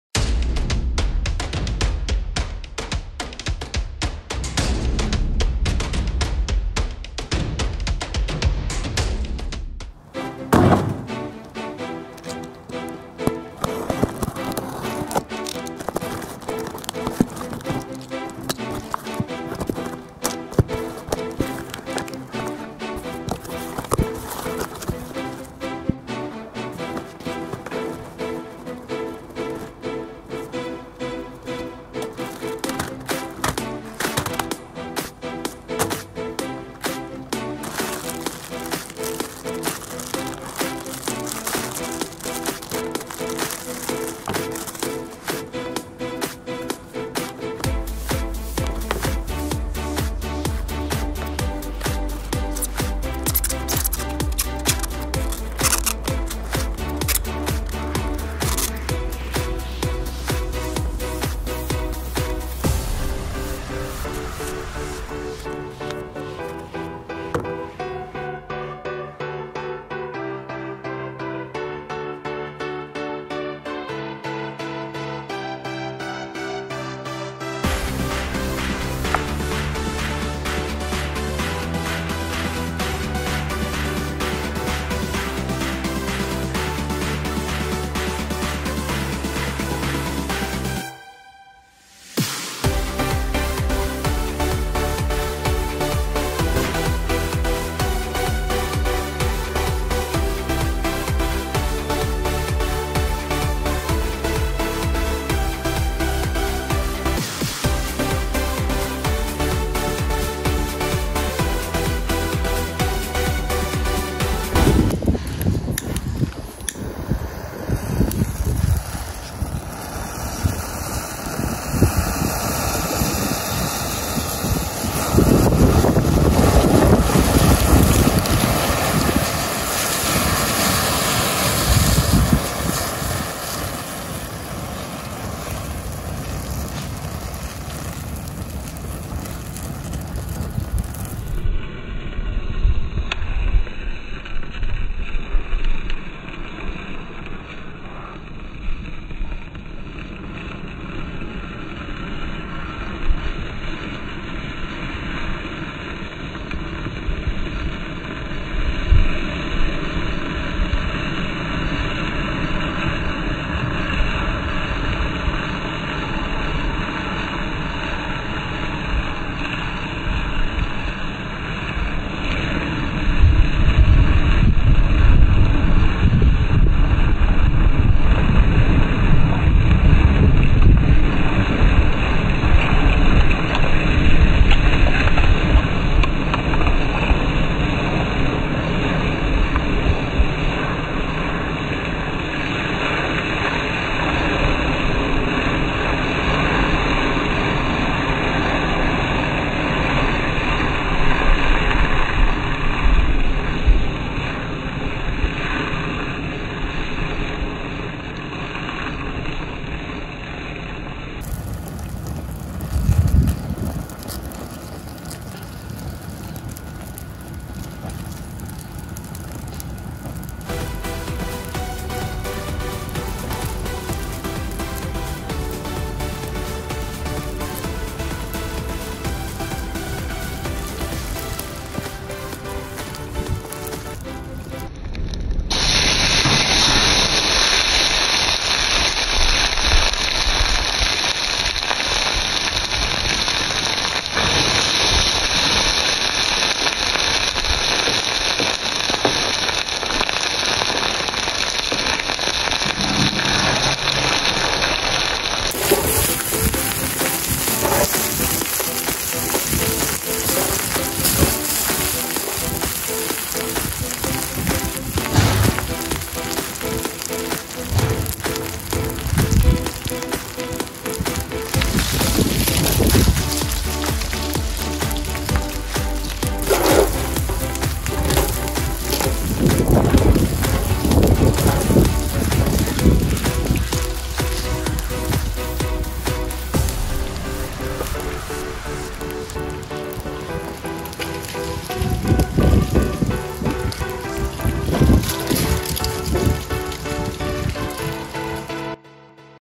Airplane Made of Matches✈️🔥
hello guys today I shot a very funny and dangerous video😁 the video is for over 18 years old. This video took me 20 days but the result was amazing.